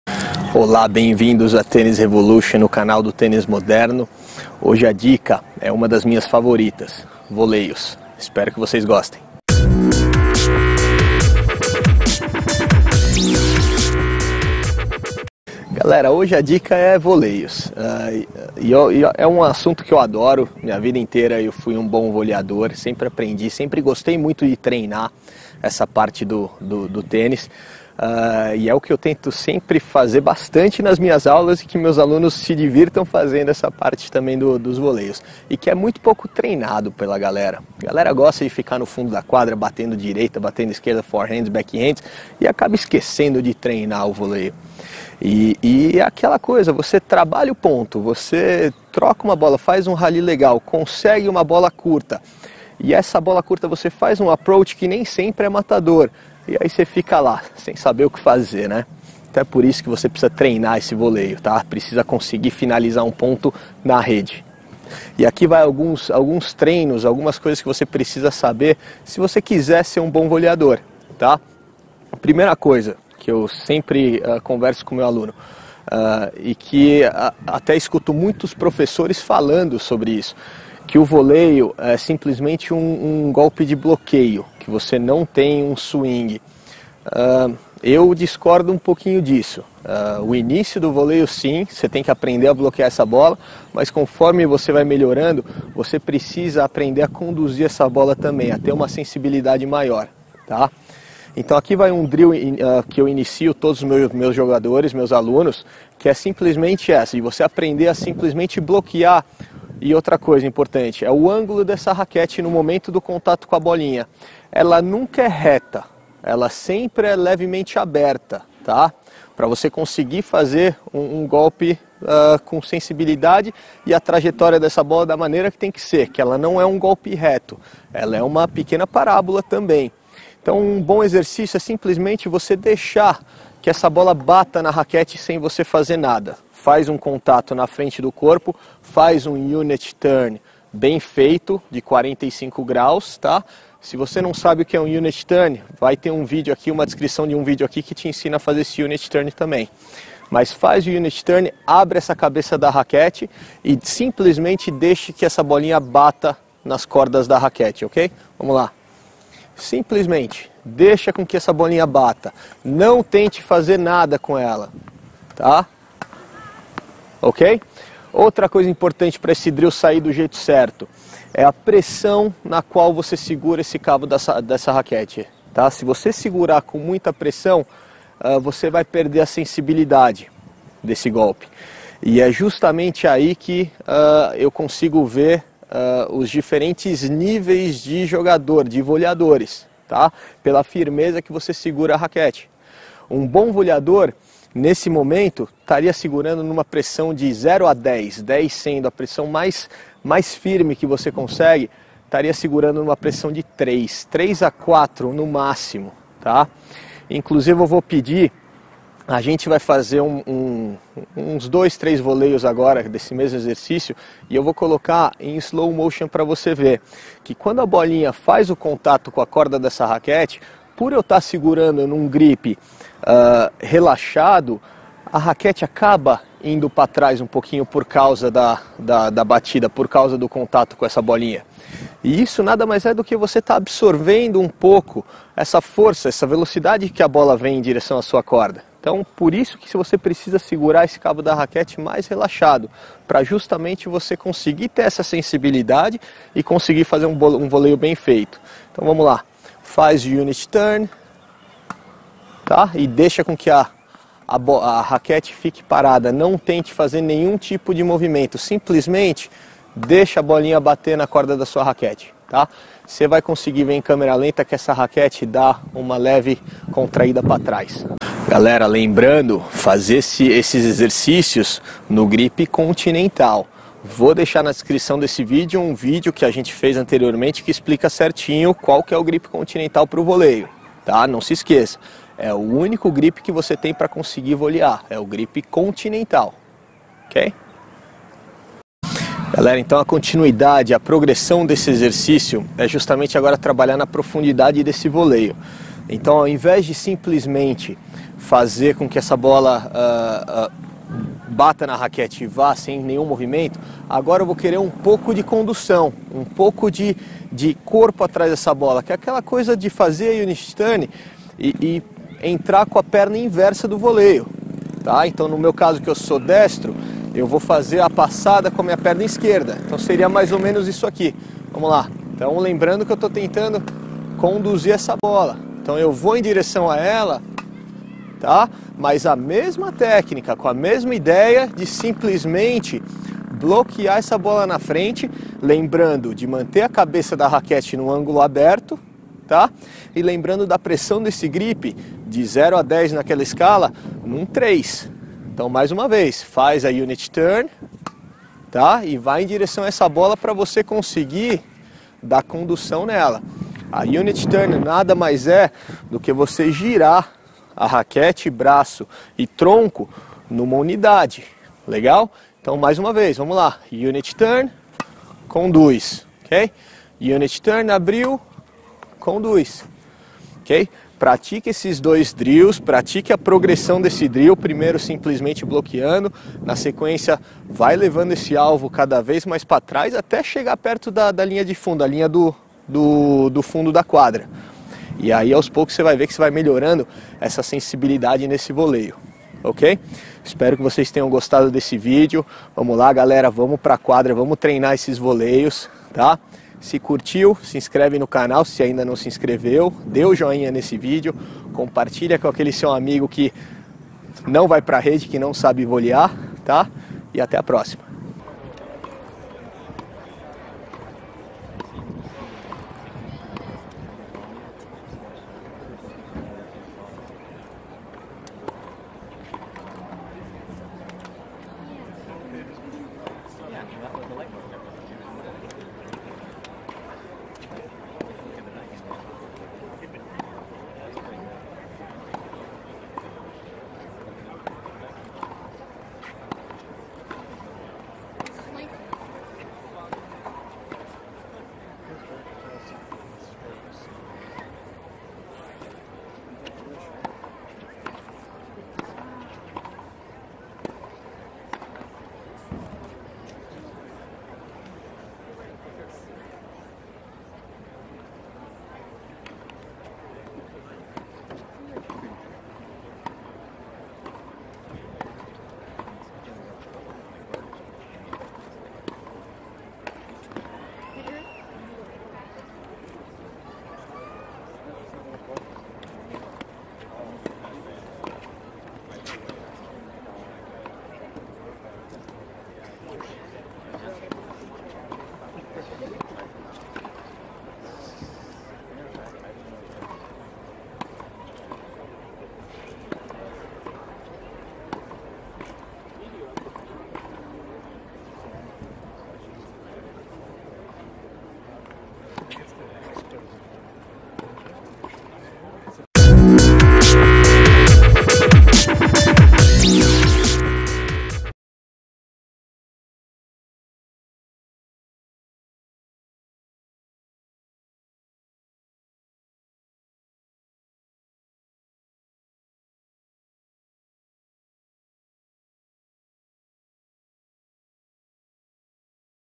🔥 Drill para um Voleio Perfeito
Você deslocou o adversário, fez um bom approach, veio à rede, o oponente chegou na bola como pôde, apenas encostou nela e ela vem voltando para o seu lado da quadra, altinha, fácil, basta apenas finalizar o ponto com um voleio simples, do lado oposto ao que seu adversário está. De repente, tudo dá errado, você encosta na bola e ela voa longe da quadra, ou então vai direto na rede, dando o ponto de graça para o rival. Há sensação mais frustrante do que isso?

🔥 Treine com a Tennis Revolution e aprenda o tênis moderno de Federer, Nadal, Djokovic e Simona Halep.

Academia: R. Santarém, 223 - Granja Viana, Cotia - SP